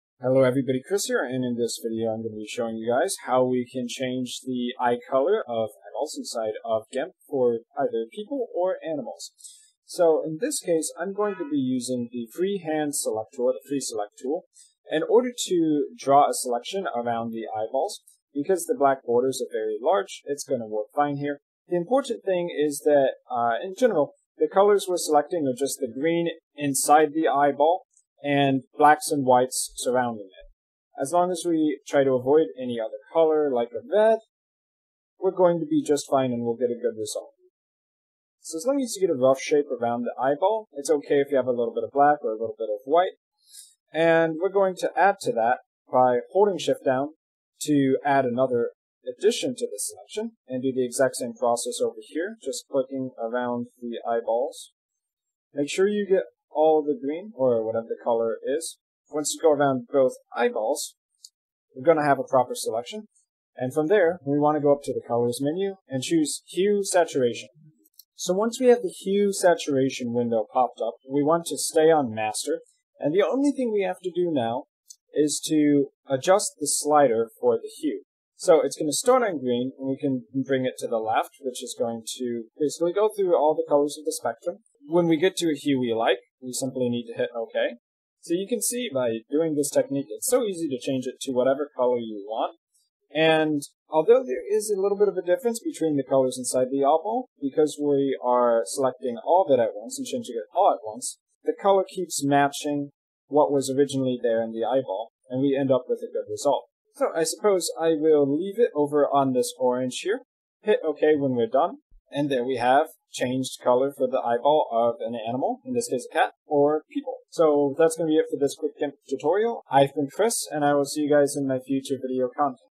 Change Eye Color Quick & Easy | GIMP Tutorial for Beginners
This tutorial shows you how you can selectively change colors of cat eyes on your photos but works for tongues, humans, etc as well

My Full Photoshop 2018 Course for Beginners - $5 OFF

Bitcoin: 17Pn5PifFmRkHRAqCZQAom1ZjCiSf7qJjJ
Ethereum: 0x6f2781F382952c8caCDbE99C46F07c265ab59627